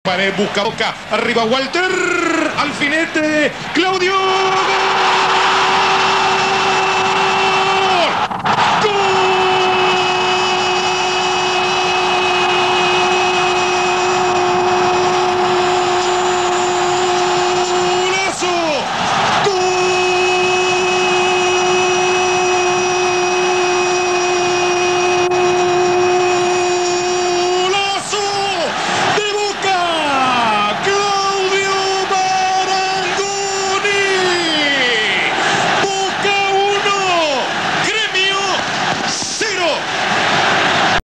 Gol de Marangoni a Gremio (Boca 2-Gremio 0 16-11-89)
Gol de Marangoni a Gremio de Brasil por la Supercopa 1989, a los 15'. (Boca 2-Gremio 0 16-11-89)